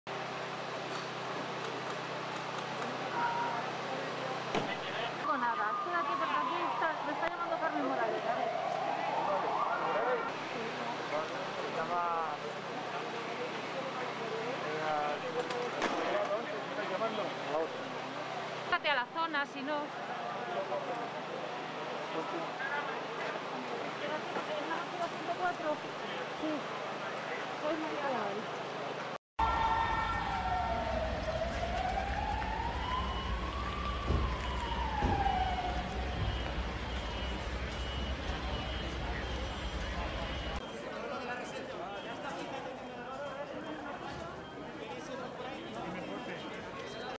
Madrid Building Explosion Leaves 2 Dead, Several Injured
BREAKING: A building explosion in Madrid, Spain, has left at least two dead and several injured. Mayor José Luis Martínez Almeida told reporters that a gas leak apparently caused the explosion, which destroyed the residential building and left other structures with mild damages. Rescuers and firefighters are searching for other possible survivors. This is a developing story.